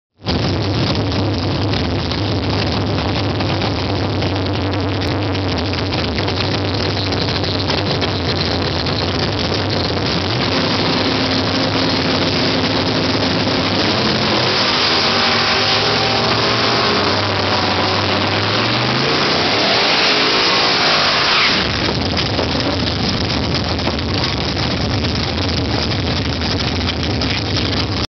St.Thomas Raceway Park - Sparta Ontario - 6/2/13
Here we have a Sunday afternoon full of races at St.Thomas Raceway Park in Sparta, Ontario.  I was up on the bridge videotaping.  I ate a lot of burnt rubber... Kind of dizzy... NHRA drag racing makes good YouTube!